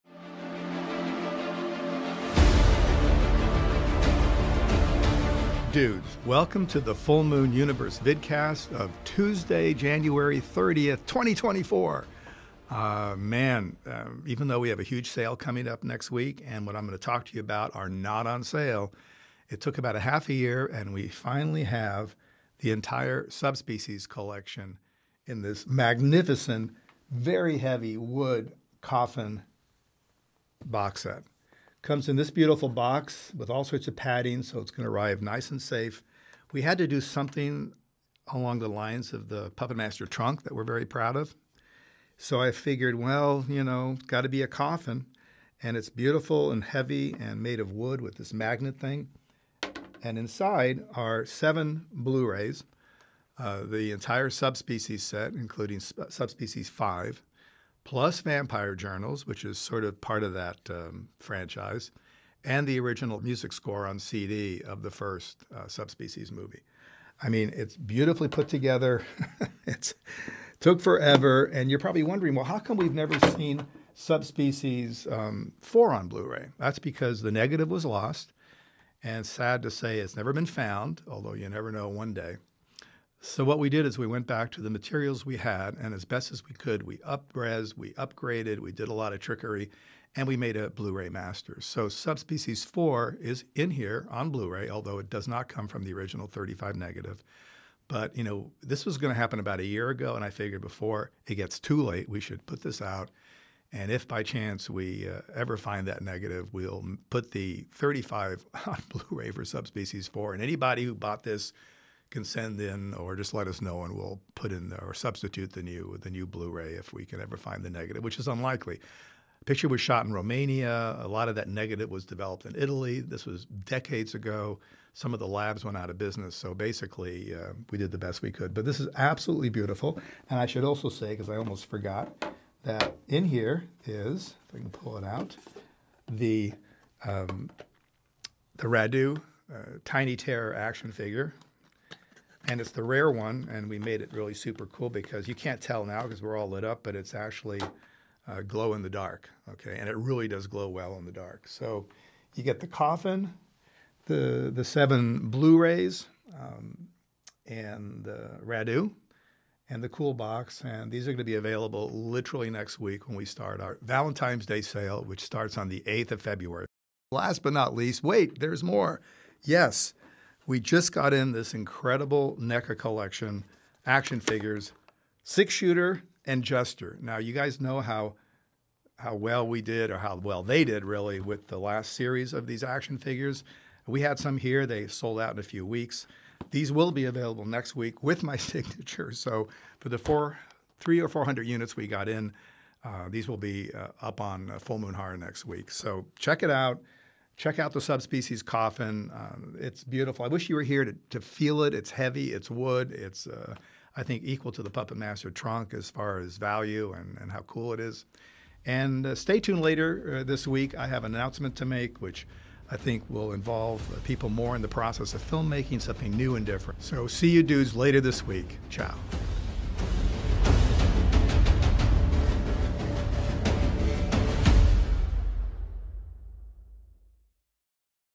Full Moon Universe | January 30th, 2024
Charles Band's Full Moon Universe rips into 2024 with a preview of some exciting new products! First, Charlie gives you your first peek at the killer upcoming SUBSPECIES "Coffin Box" Blu-ray box set! Then, get an eyeful of NECA's latest PUPPET MASTER figures! Plus, Charlie hints at some other stuff brewing at FM HQ! Don't miss it!